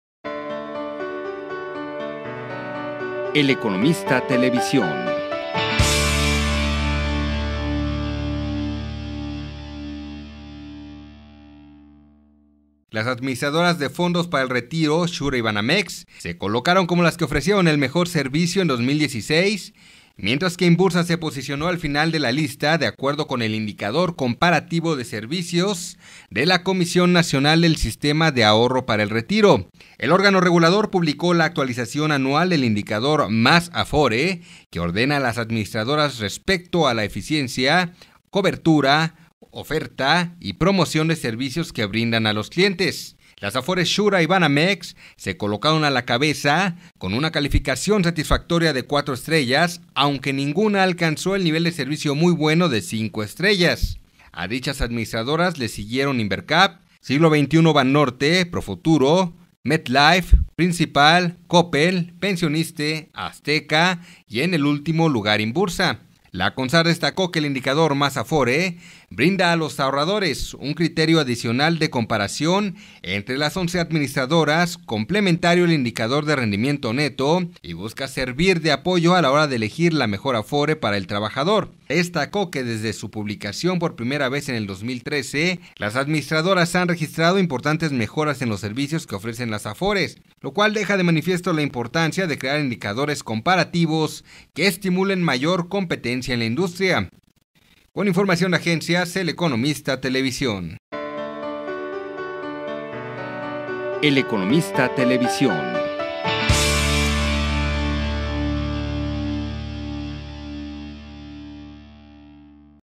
Sura y Banamex, las mejores Afores del 2016
Las Administradoras de Fondos para el Retiro Sura y Banamex se colocaron como las que ofrecieron el mejor servicio en 2016, mientras que Inbursa se posicionó al final de la lista, de acuerdo con el indicador comparativo de servicios de la Comisión Nacional del Sistema de Ahorro para el Retiro.